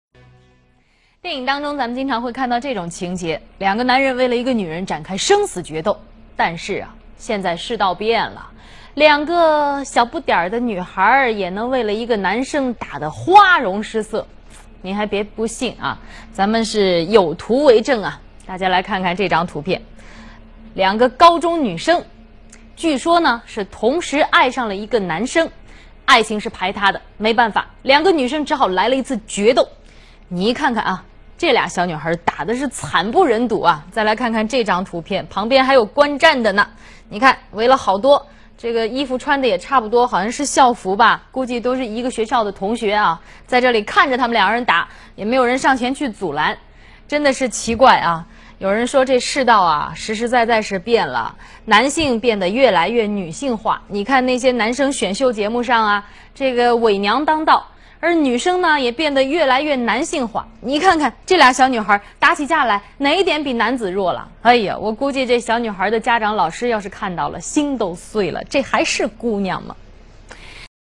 Pinch and Zoom on Chinese TV
Pinch and Zoom on Chinese TV. Apple can sue everyone under the sun but the Chinese won't give a damn!